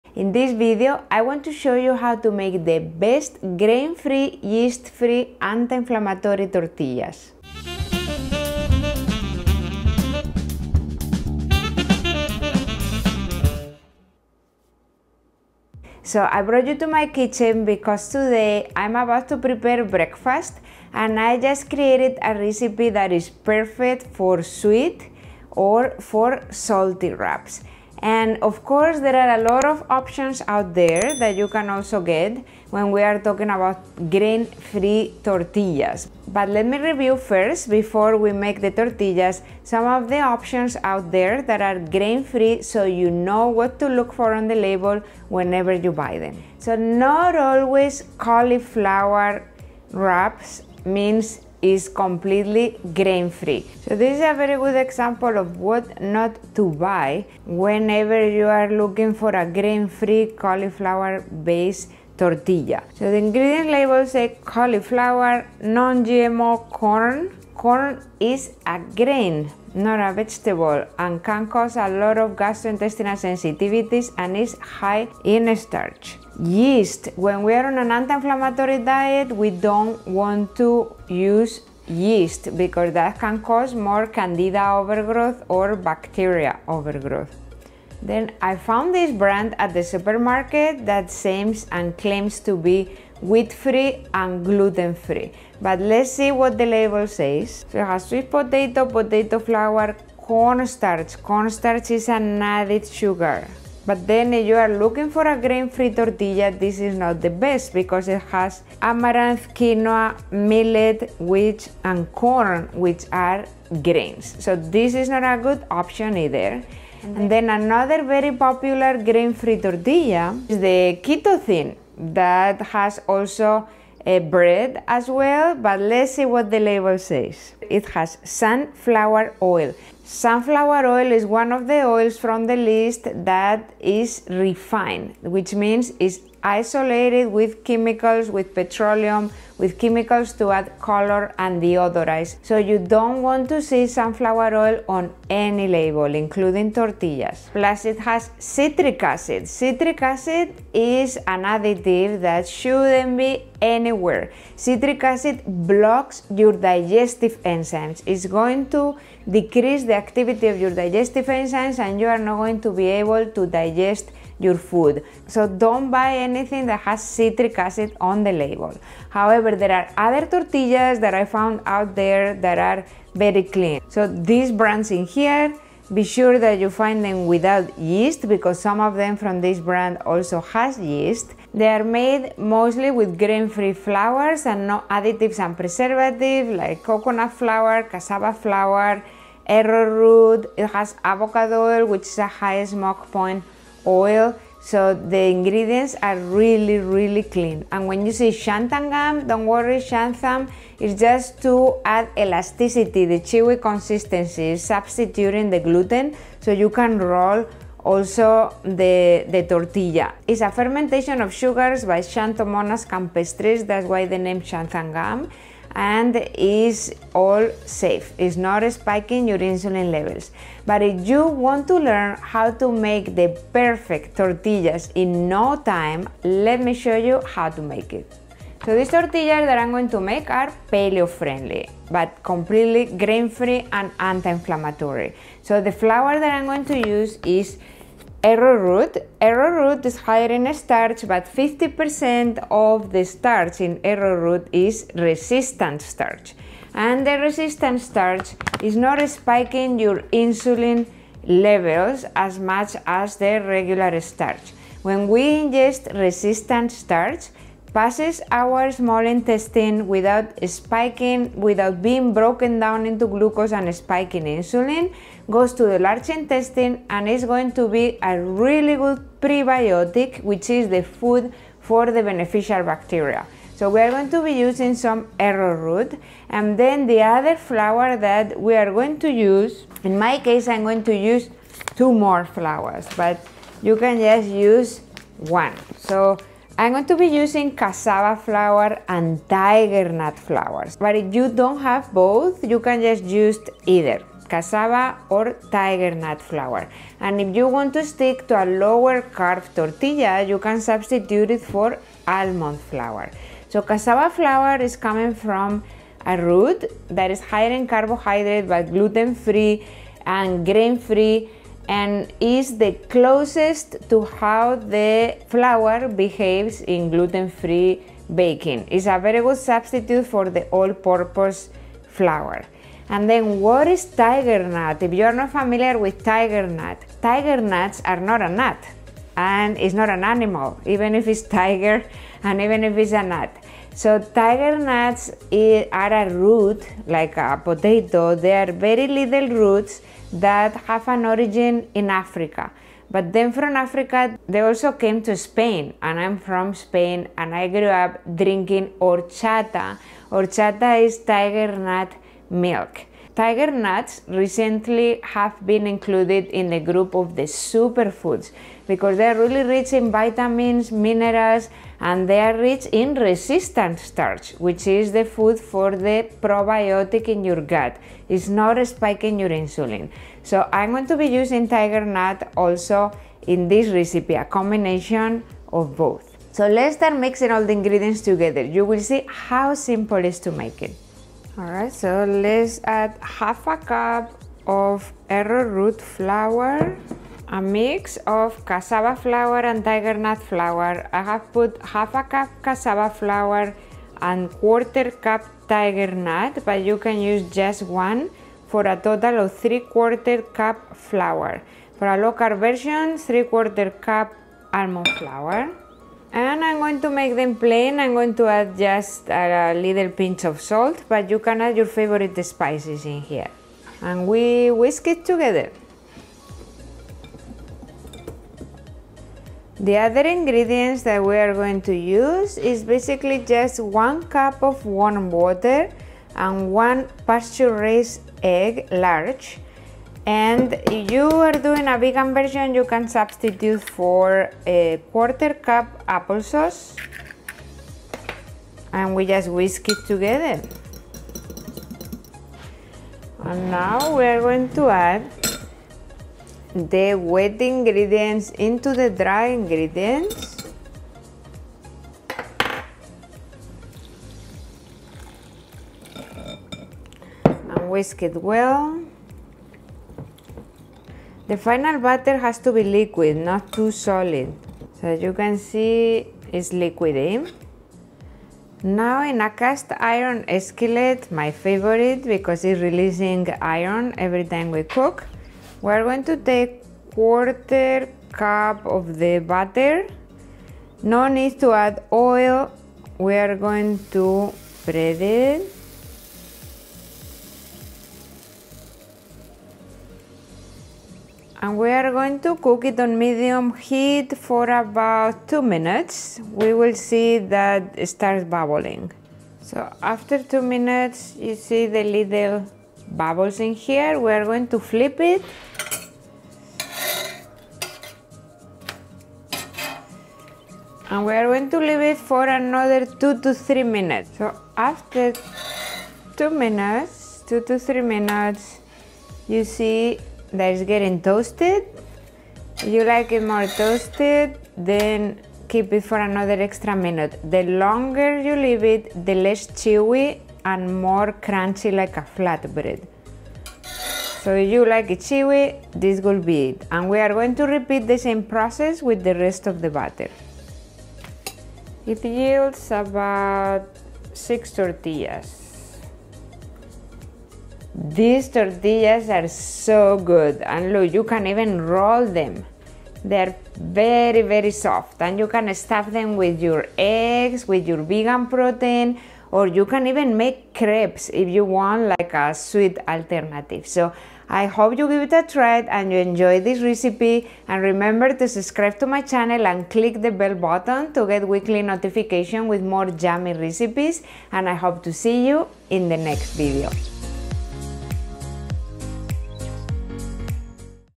How to Make Grain Free Soft Tortillas | Cassava or Almond Flour Tortillas | Store Wraps Review
Delicious Grain Free Soft Tortillas Recipe that you can use for your favorite sweet or salty fillings. I show you the option for paleo or low carb (cassava or almond flour tortillas) alternative. The paleo version is FODMAP friendly.
I also show how to read the label in some of the most common grain free tortilla wraps at the store (Store wraps review).

Easy, simple and tasty.

👇ITEMS USED IN THIS RECIPE👇
Cassava flour
Arrowroot flour
Tiger nut flour
Cast iron griddle

👉Get Elena's book "Ketorranean Diet Solution" now available here:
- hard copy